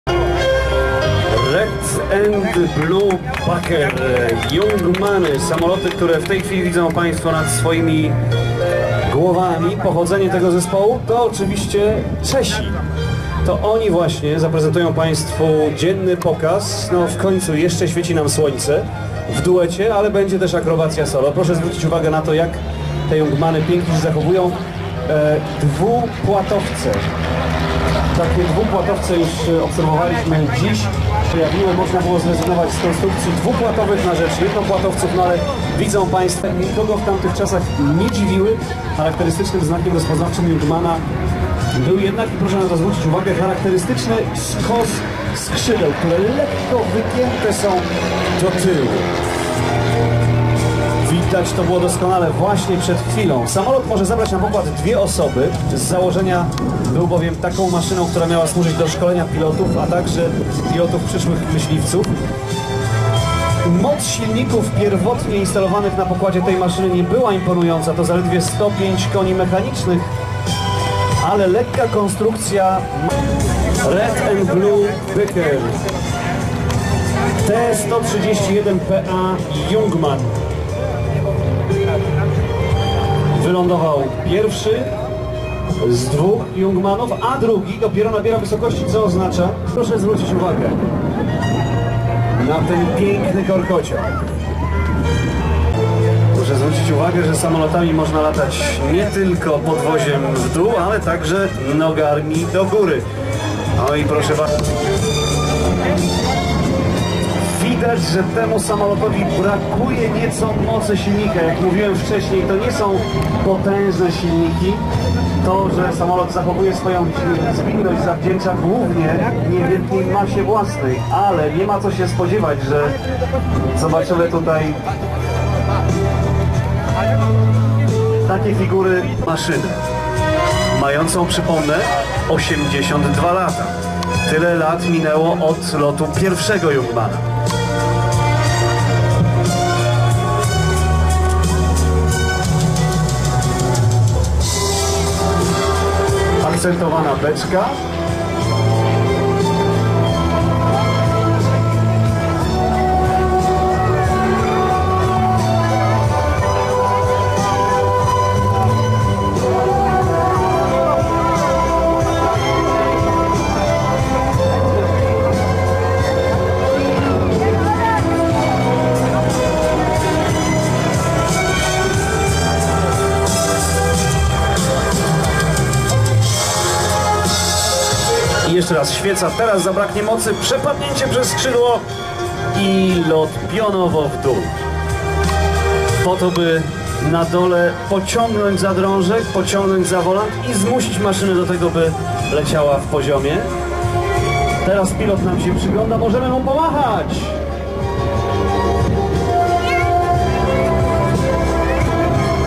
Red and Blue Bücker Leszno 2016
Amatorskie nagranie z pokazów lotniczych zespołu Red and Blue Bücker na samolotach Jungmann T-131 PA,  Leszno 2016.